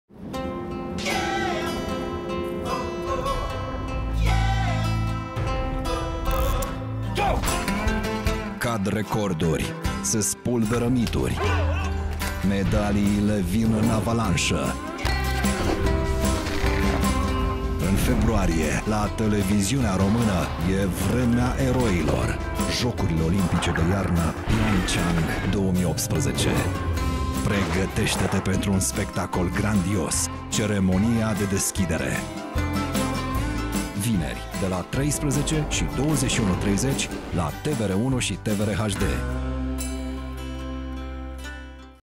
Ceremonia de deschidere a Jocurilor Olimpice de Iarnă PyeongChang 2018, în direct la TVR1
Vineri, 9 februarie, în direct la TVR1 şi TVR HD vedem împreună Ceremonia de deschidere a Jocurilor Olimpice de Iarnă PyeongChang 2018. Biatlonistul Marius Petru Ungureanu va fi purtătorul de drapel al României. Delegaţia ţării noastre este formată din 28 de sportivi, dintre care o rezervă, şi 27 de tehnicieni şi oficiali.